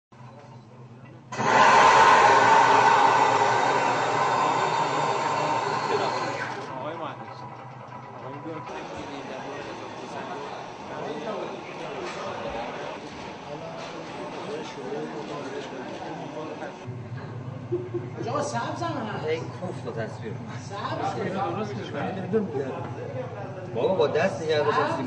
IR.Iran unveils heavy diesel engine project
President Mahmoud Ahmadinejad unveiled the first national heavy diesel engine on Thursday. During a trip to northern Mazandaran province, Ahmadinejad visited Iran's heavy diesel industrial company (DESA) working to run the engine since 2005.